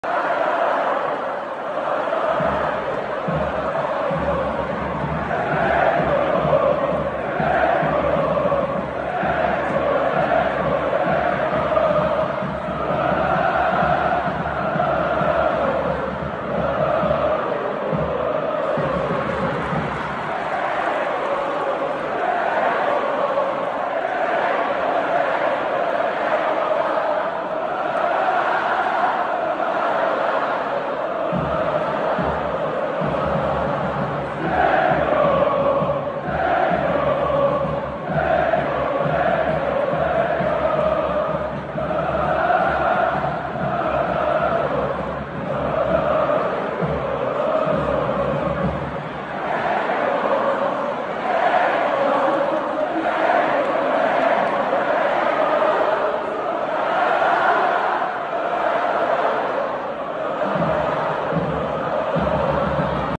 Dzieci na Żylecie dały czadu🔥🔥🧨 Legia-Molde
Doping dziecięcych grup kibicowskich na meczu Legia-Molde. Dzieci zasiadły na Żylecie